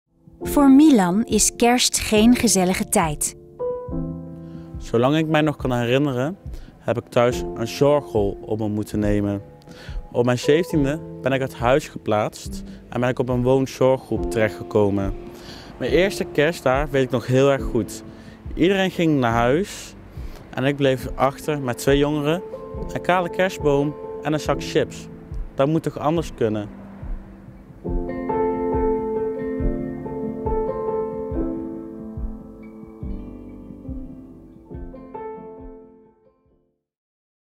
Het Vergeten Kind Kerst 2021 - Unforgettable Milan
Milan voelde zich altijd heel eenzaam met kerst. Hij zet zich nu in voor vergeten kinderen in Nederland - Help jij mee om deze kinderen een Onvergetelijke Kerst te bezorgen?